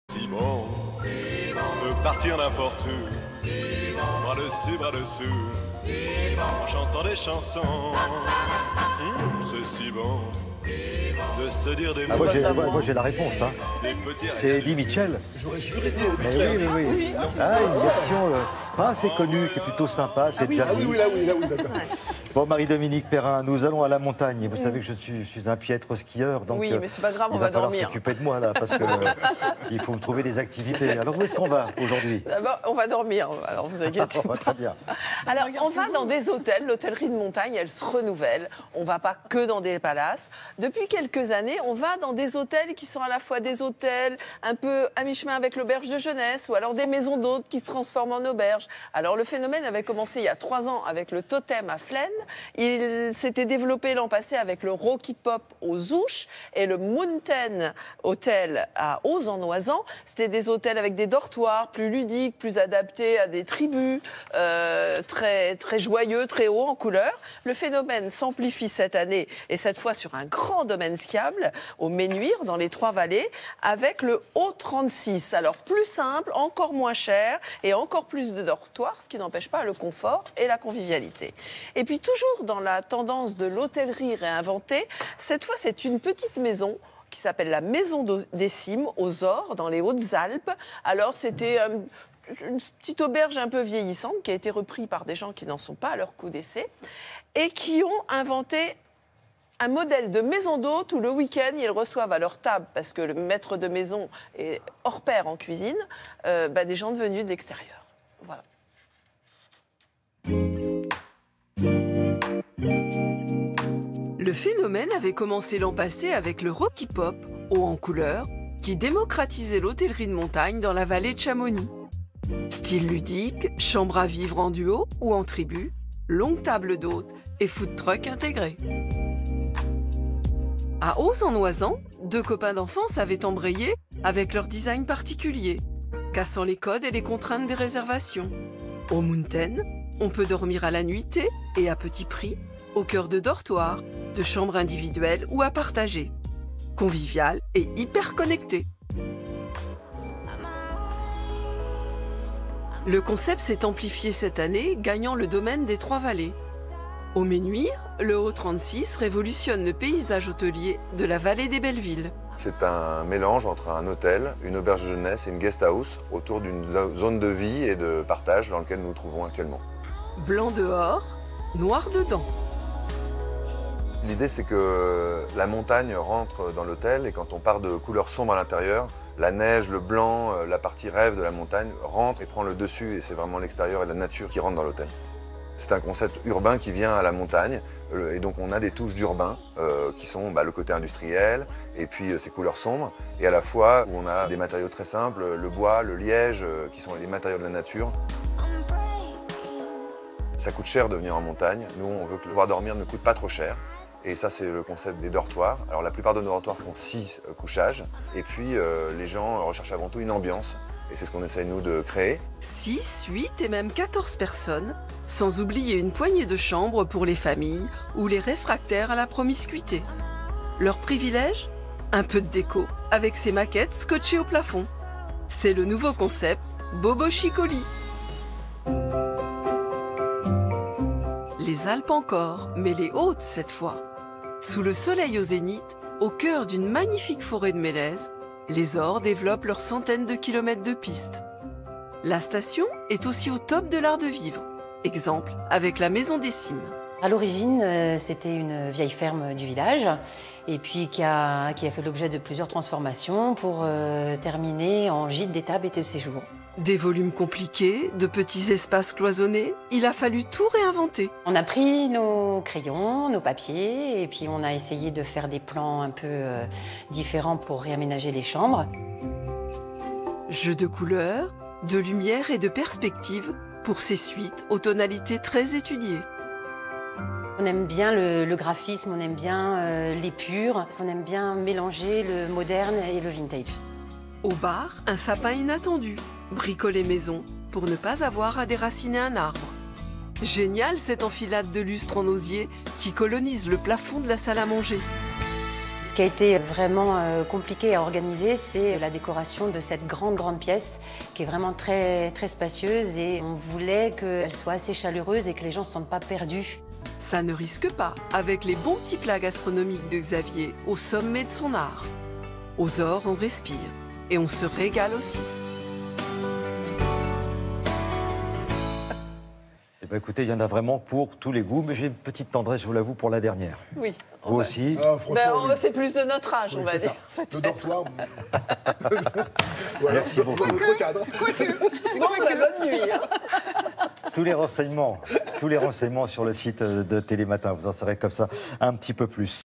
Made in France - Tendance neige
Le phénomène avait débuté l’an passé avec des établissements qui avaient fait le pari de démocratiser l’hôtellerie de montagne…
Aux Houches en Haute-Savoie, à Oz en Oisans en Isère, en dormant soit en chambre particulière ou familiale, soit en dortoir pour 6/8 personnes…
Le phénomène s’amplifie cette année avec une nouvelle adresse aux Ménuires en Savoie…
Toujours dans la tendance d’une hôtellerie réinventée, version chambres d’hôtes mais avec restaurant intégré, c’est aux Orres dans Les Hautes-Alpes.